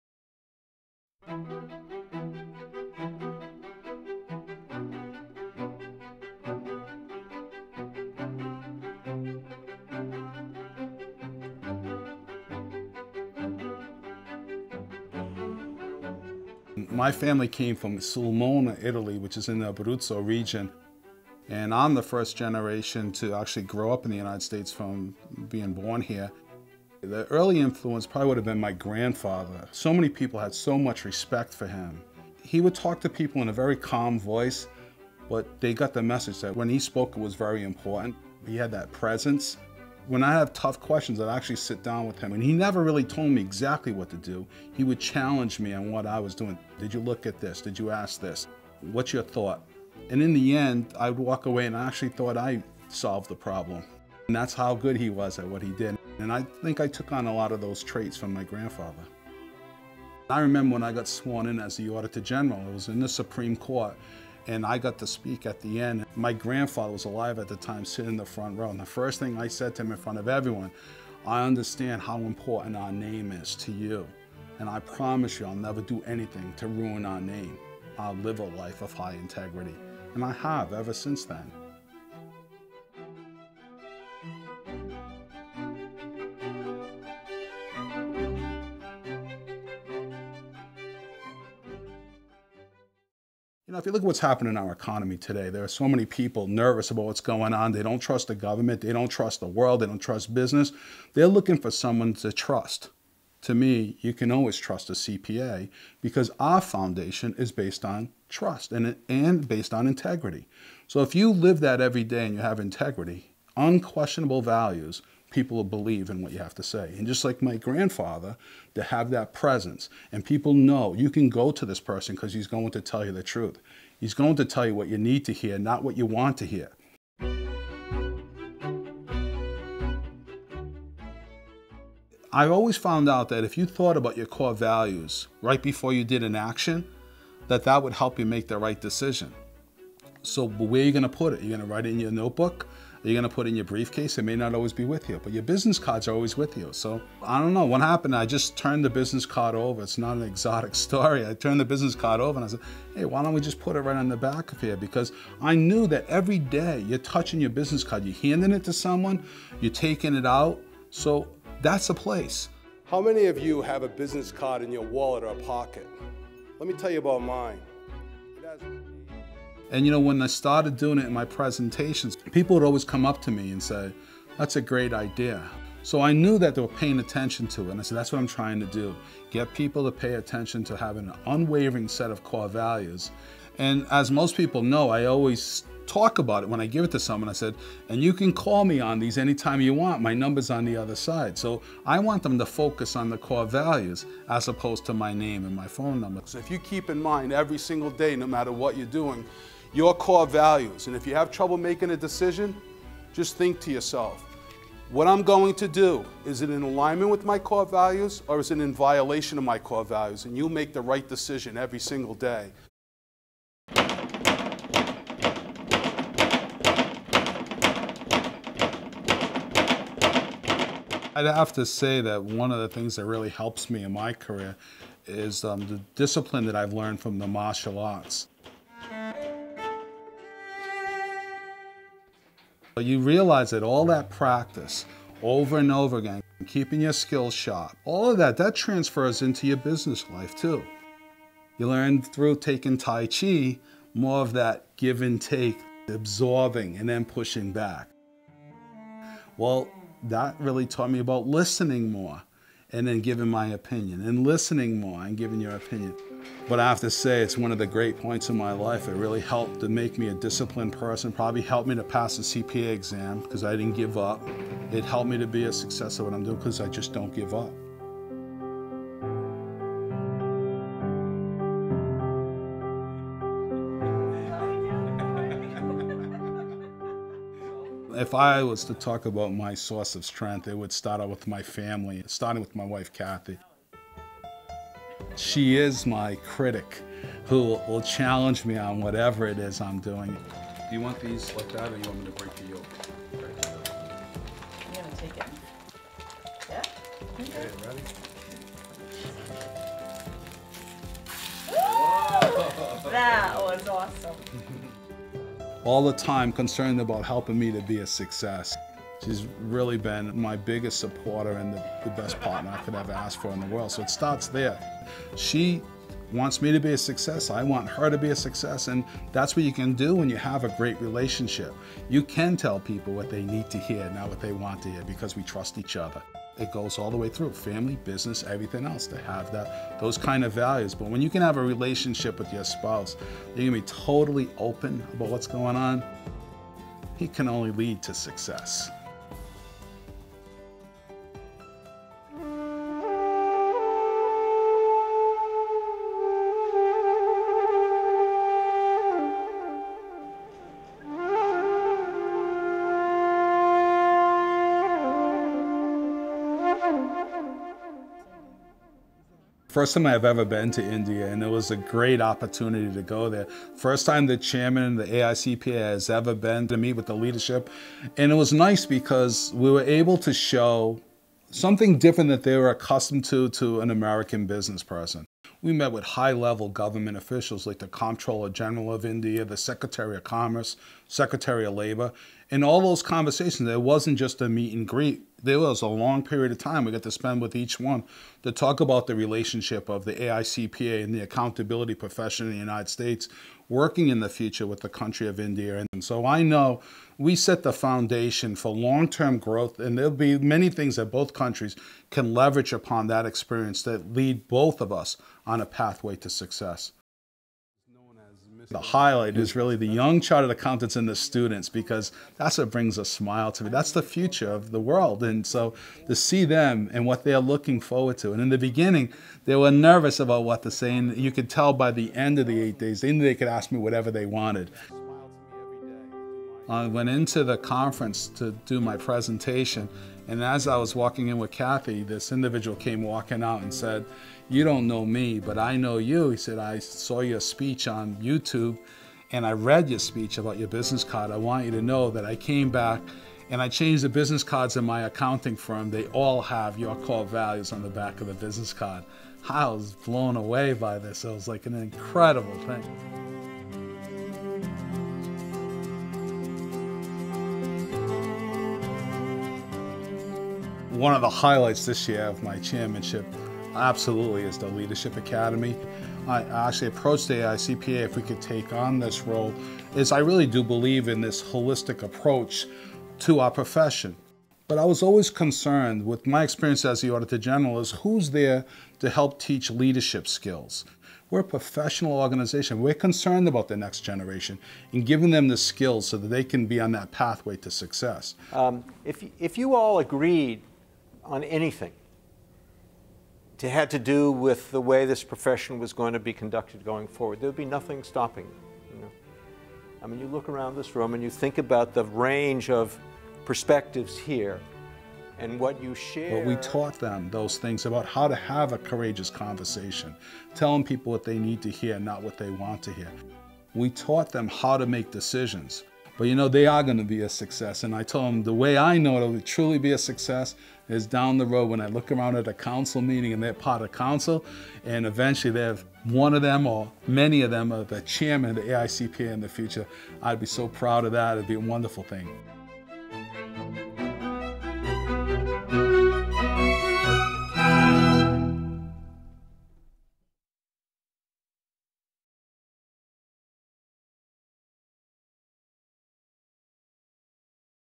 Ernie Almonte, CPA: "A Day in the Life"
Inspiring leadership in his professional and personal life, it is the core values of integrity, reliability, independence and accountability that guide former AICPA Chairman (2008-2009)/Auditor General of the State of Rhode Island, Ernie Almonte, CPA